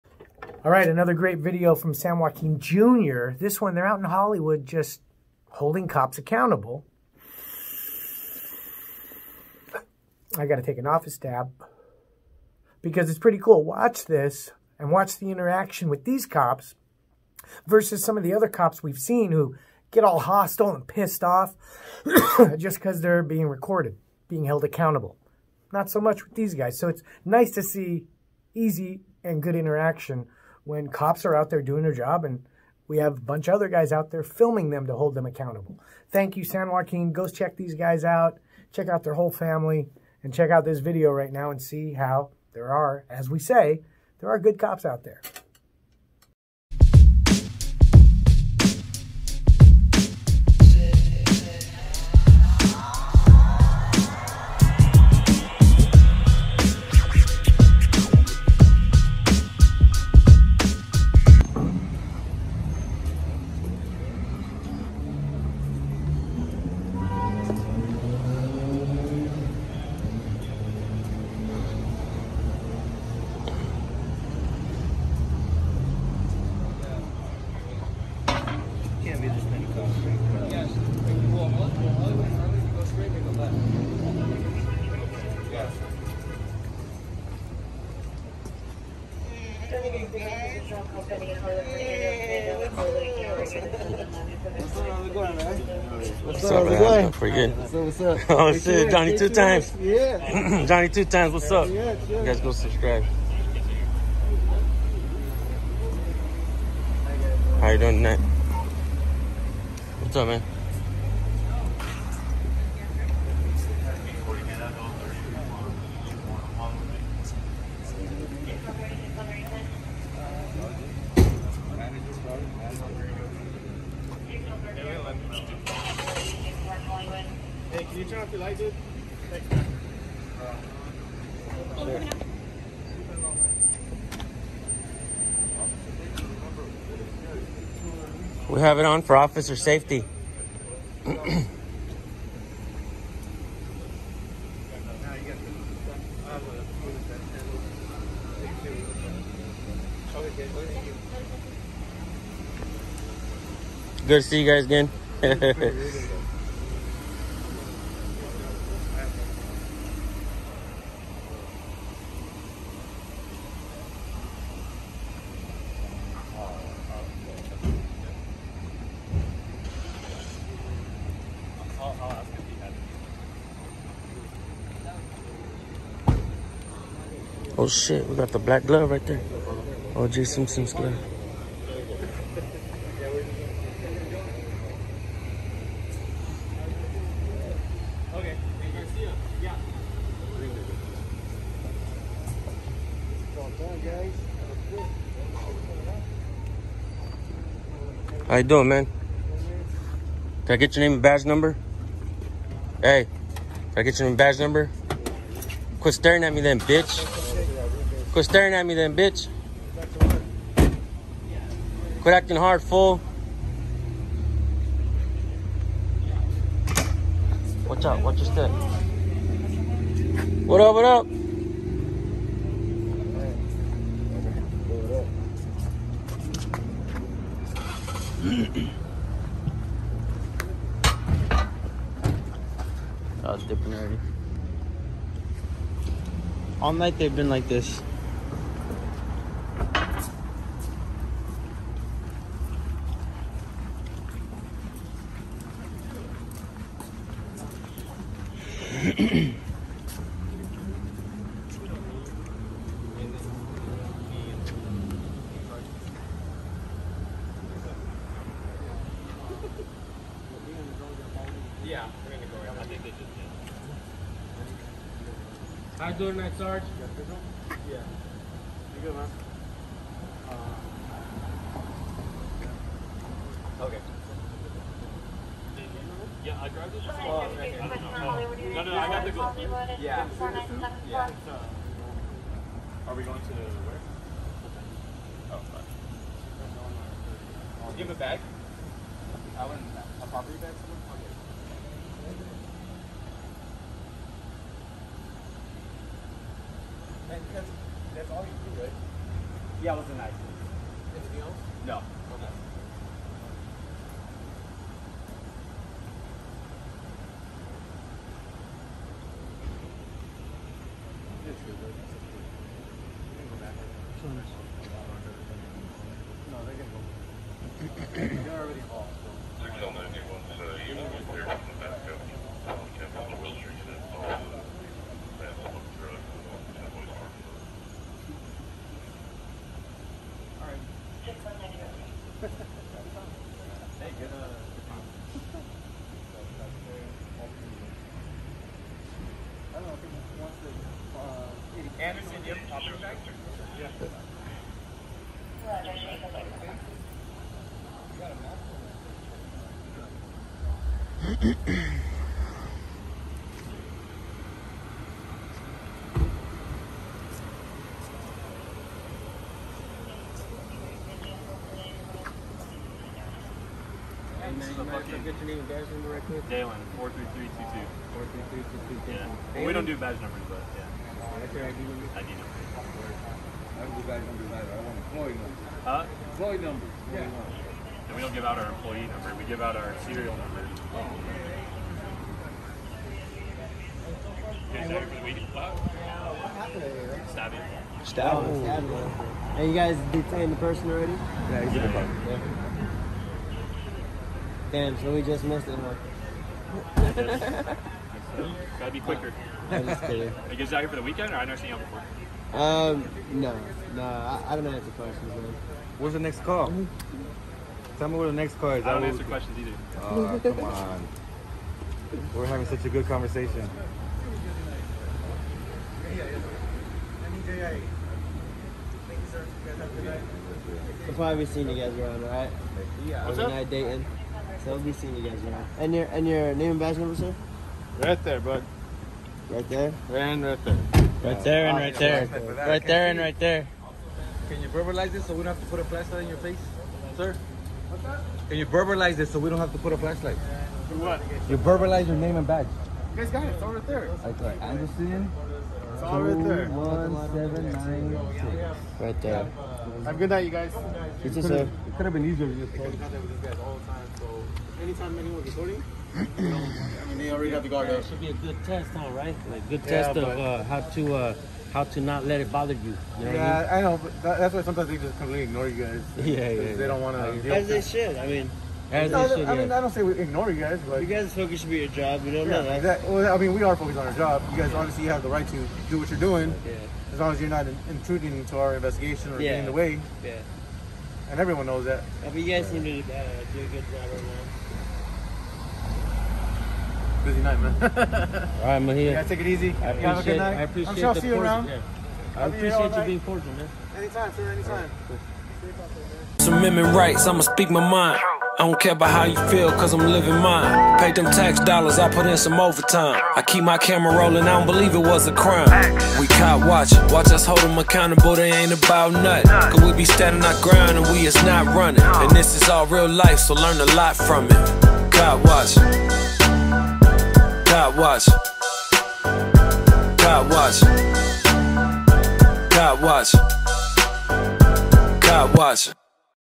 holding cops accountable Always film Police
even when nothing is going on we MUST record the police. Perhaps because they were there filming, the cops watched themselves. nice to see interaction NOT become hostile. Some cops still want to have a HARD attitude but a lot are getting used to the fact that citizens are stepping up and pulling out those cameras.